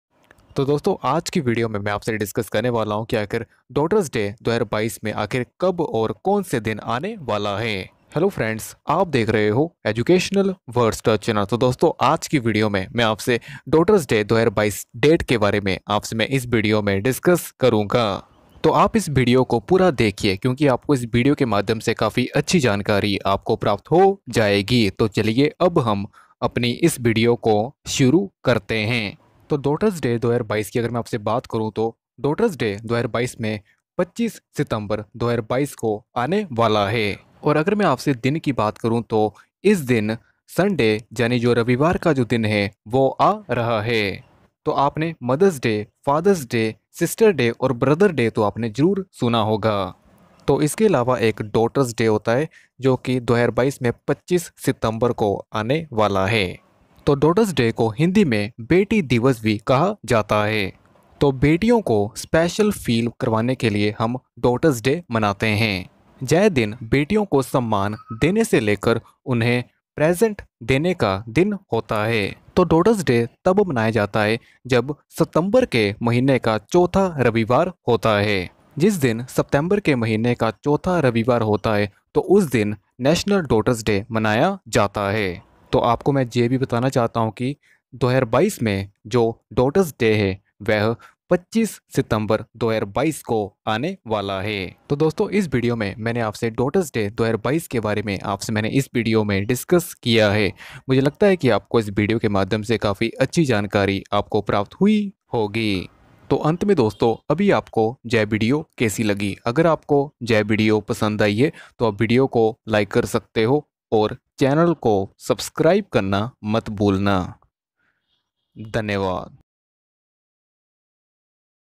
Daughters Day 2022 Date | बेटी दिवस 2022 में कब है | 25 September 2022 | In Hindi
Hi,

In this video i will discuss about Daughters Day 2022 Date.